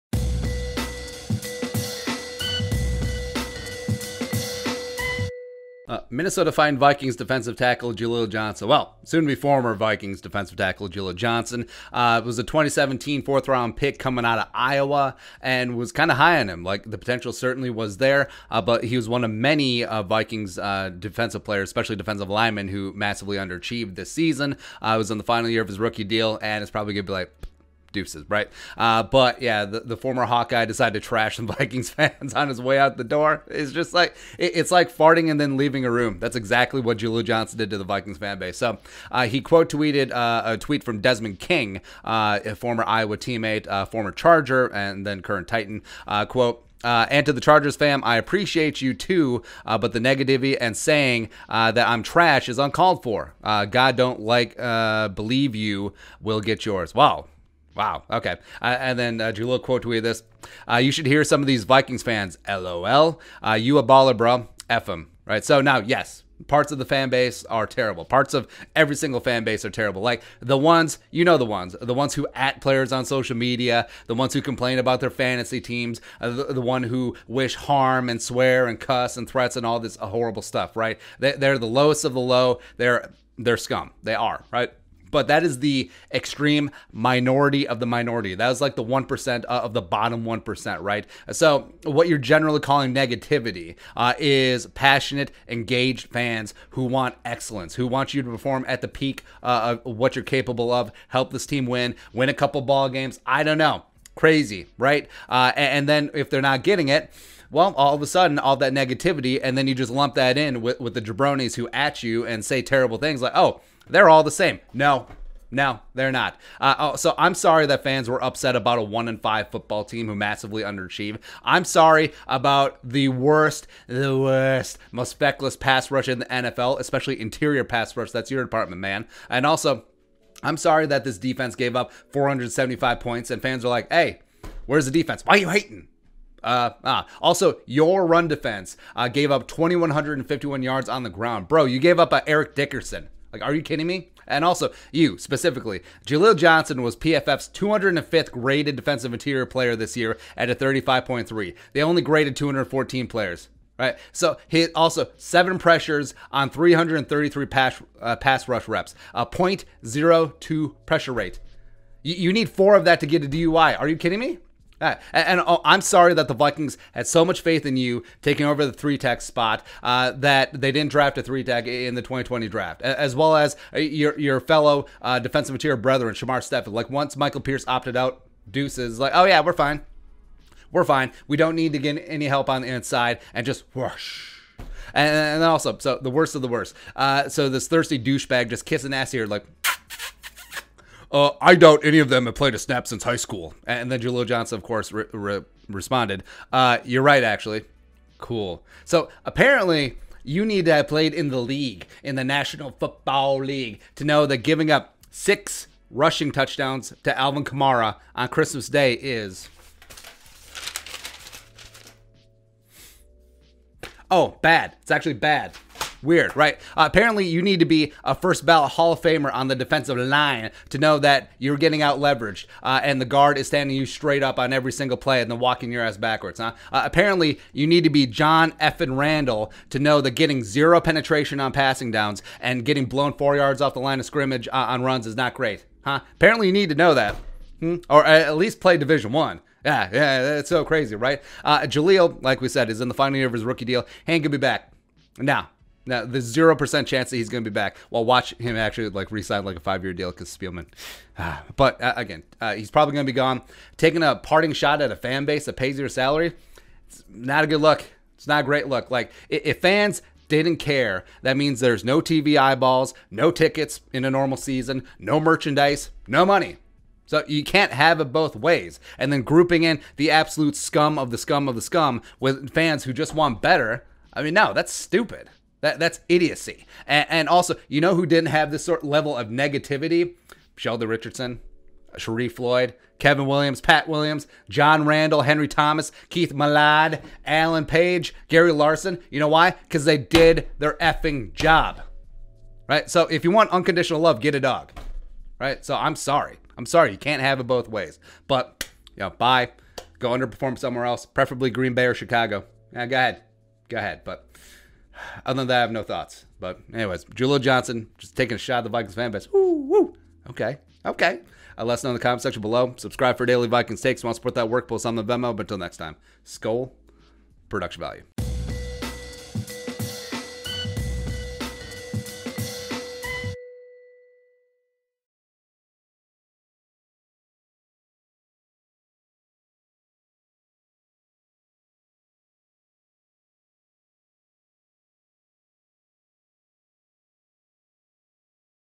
Jaleel Johnson Trashes Vikings Fans on Twitter 🚮🚮🚮
Soon to be former Minnesota Vikings defensive tackle Jaleel Johnson trashed Vikings fans on Twitter for being too negative about his horrible level of play. Cool.